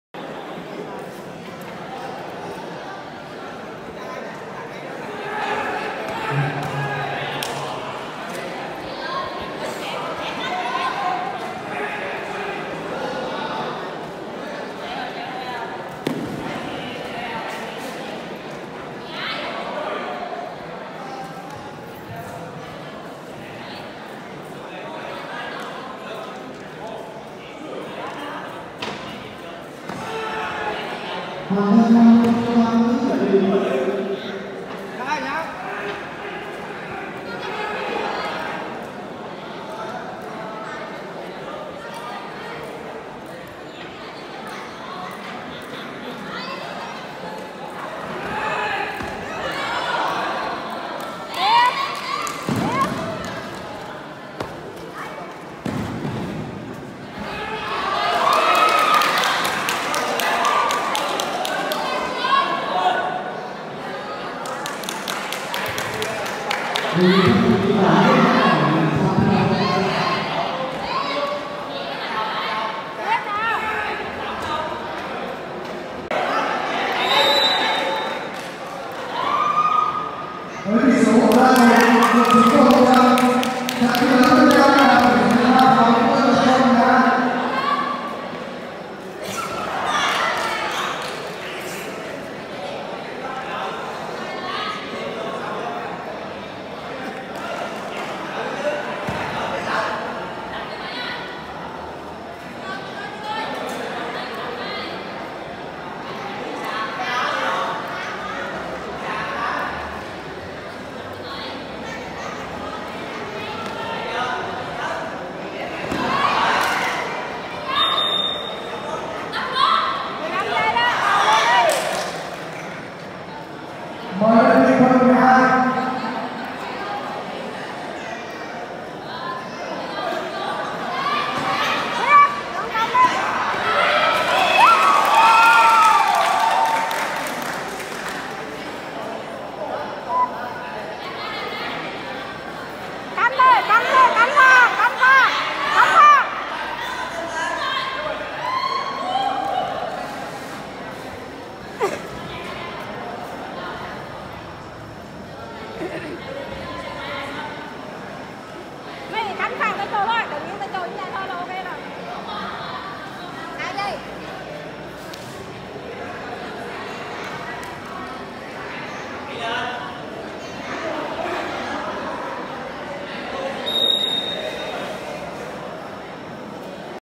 Bán kết 45kg nữ. Aka Thanh Như, ĐH TDTT K38 - Ao Phan Thị Hồng Mỹ, Tanmido
Giải vô địch Karate ĐH TDTT - TP HCM mở rộng 19/5/2018